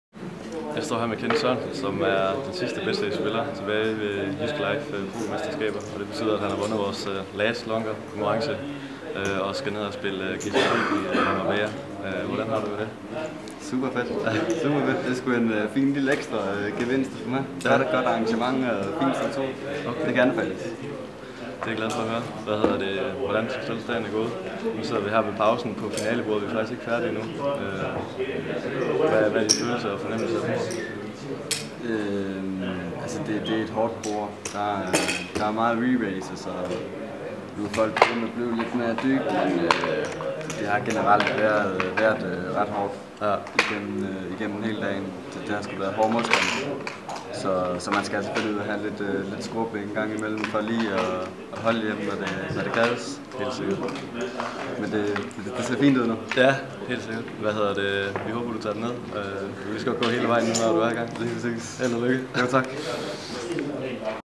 Jyske mesterskaber i poker - Last longer vinder - Kenneth Sørensen
Kenneth Sørensen blev nr. 3 til de Jyske Mesterskaber i poker. Udover præmien på 100.310 kr. fik Kenneth også et buy-in til GSOP Marbella, til en værdi af $1.500, som belønning for at være den sidste Betsafe spiller til at forlade turneringen.